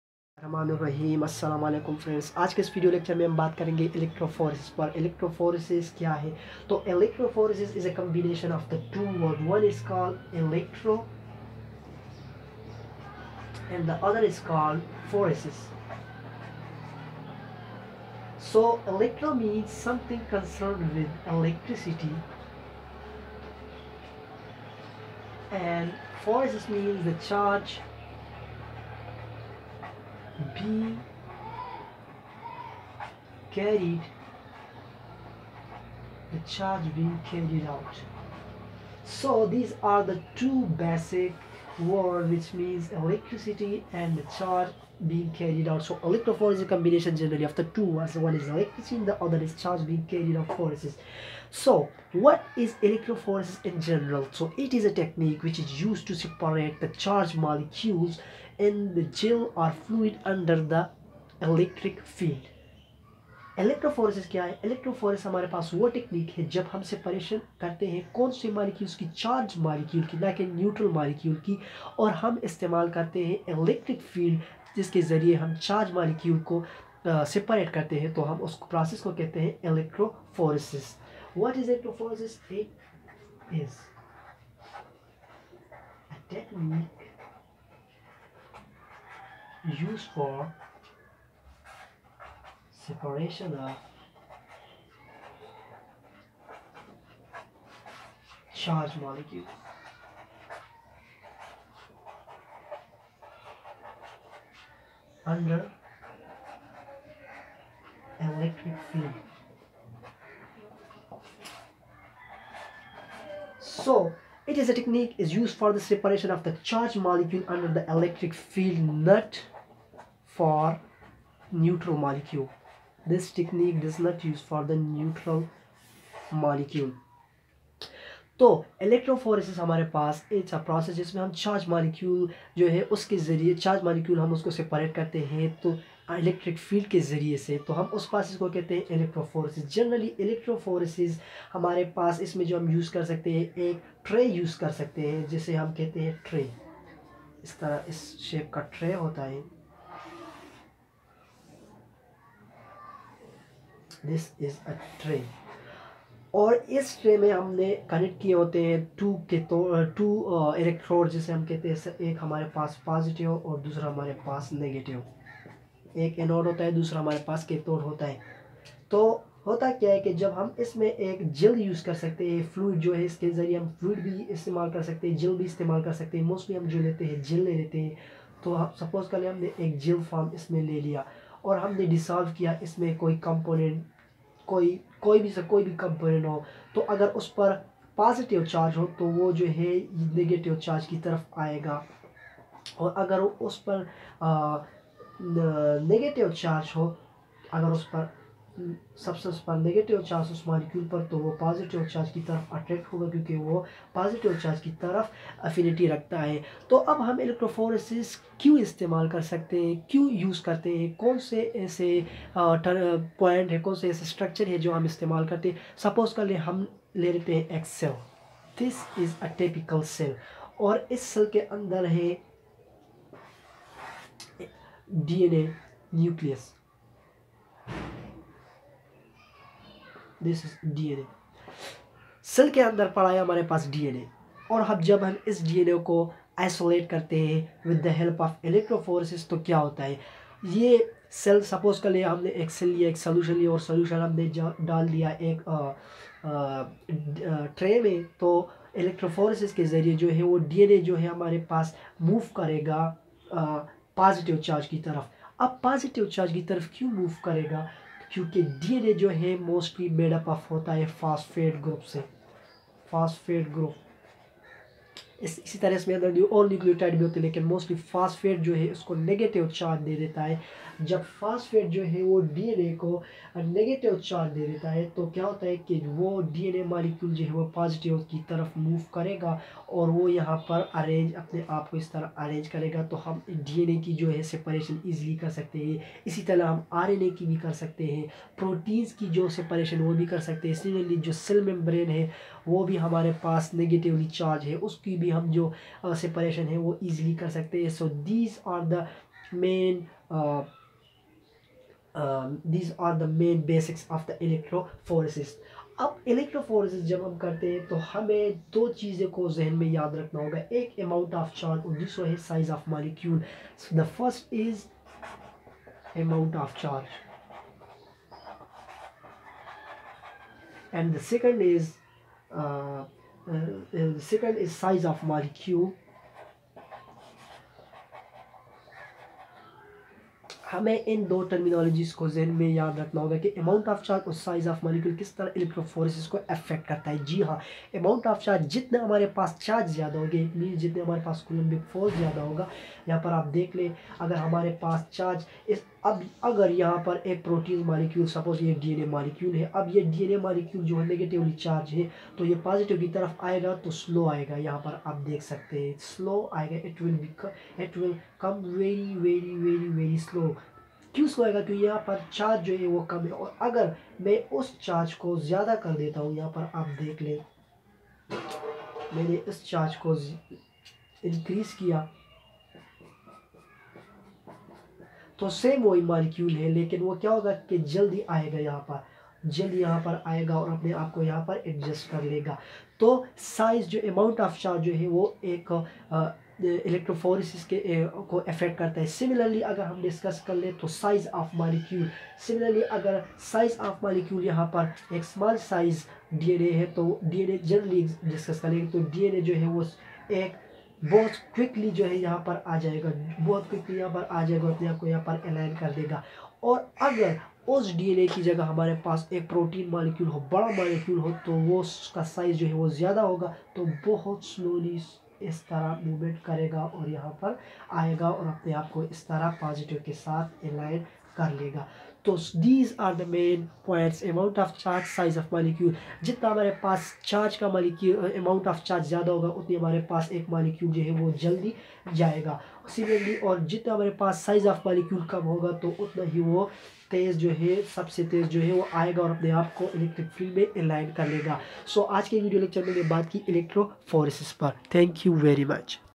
Electrophoresis for Etea by AFAQ AHMAD COACHING ACADEMY
In this video i explained the basics of electrophoresis and why we use it for charged molecule separation. i have also touched a little jel elecrophoresis for the sake of concept for beginners in this video lecture i have also explained that how size and charge of molecule effectthe process of electrophoresis .
AFAQ AHMAD COACHING ACADEMY